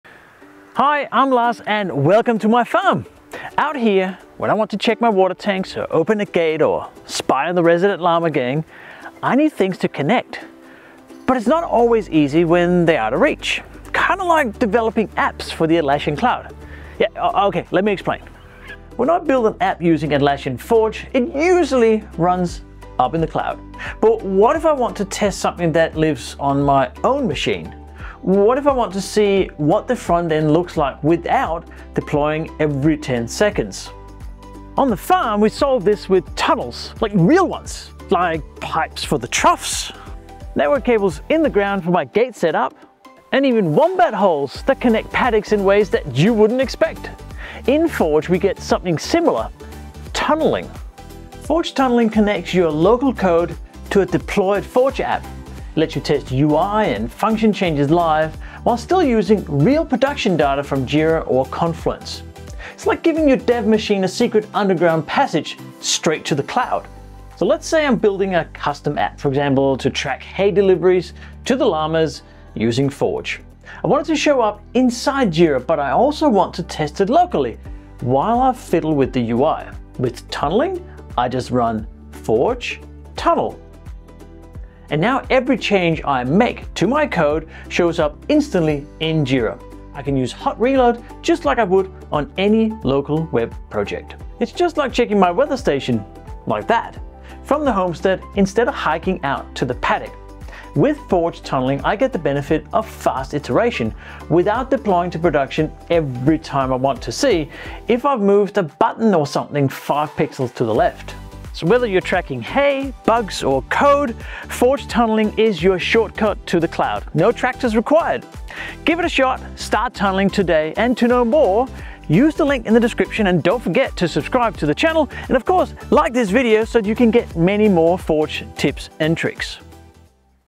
What is a Forge Tunnel? When should you use it?
The Forge Tunnel is one of those features that speeds up your development workflow and lets you use the best of developing locally for a cloud app.